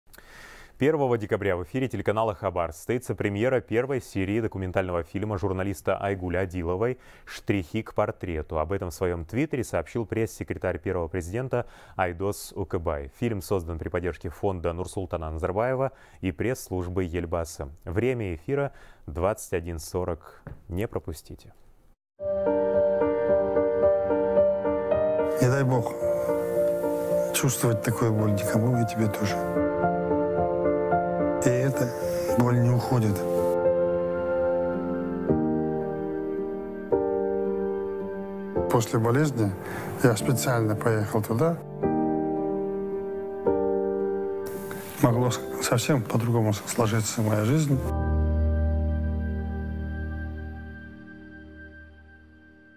Премьера документального фильма о жизни и деятельности Елбасы состоится на «Хабаре»
Первого декабря в 21:40 в эфире телеканала «Хабар» состоится премьера первой серии документального фильма журналиста Айгуль Адиловой «Штрихи к портрету», созданного при поддержке Пресс-службы Елбасы и Фонда Нурсултана Назарбаева.